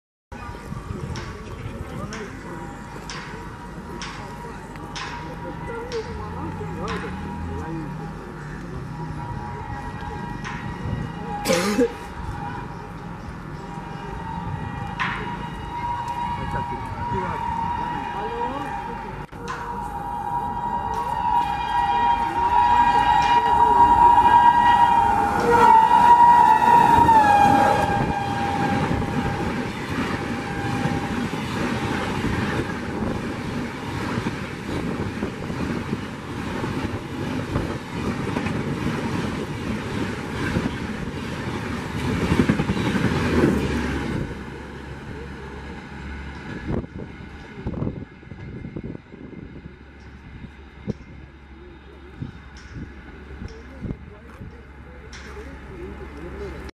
Late Running - 12424 DIBRUGARH RAJDHANI EXPRESS !!
12424 New Delhi - Dibrugarh Town Rajdhani Express hauled by GZB WAP-7 30208 crossing Dildarnagar Junction at high speed running late by 8 hours .